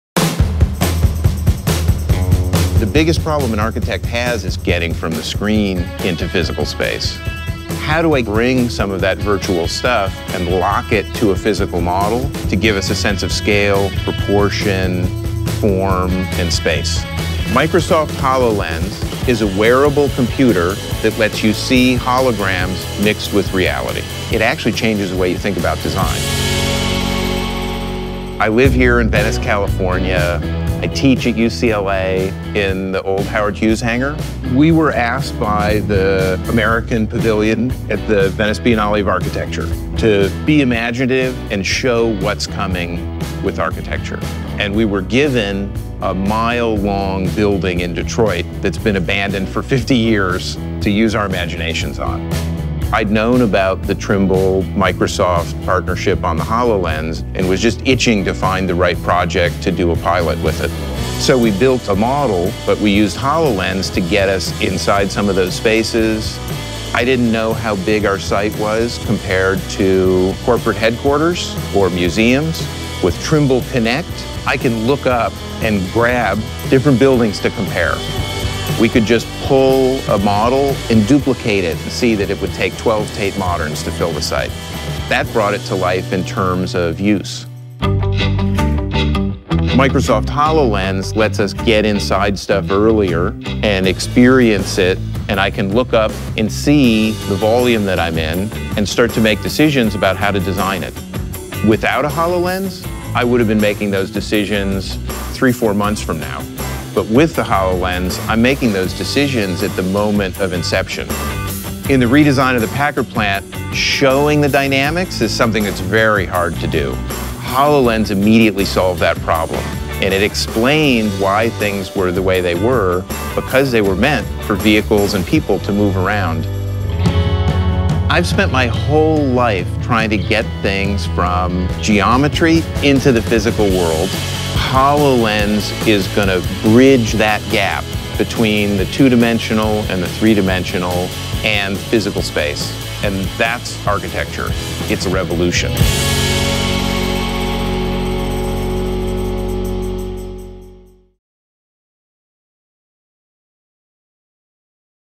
홀로렌즈 파트너, 건축가 Greg Lynn 이야기
건축가 Greg Lynn이 전하는 홀로렌즈와 건축 이야기